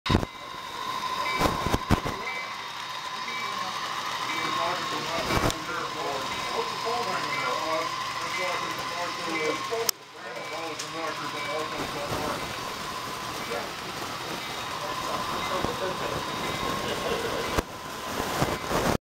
Fellow club member Frank's Oak River R R former CSX EMD "Patch" unit.
This video is from the Oak Park Society Of Model Engineers,H.O Scale Model Railroad Club in Oak Park Illinois. Wednesday night, September 28th, 2022.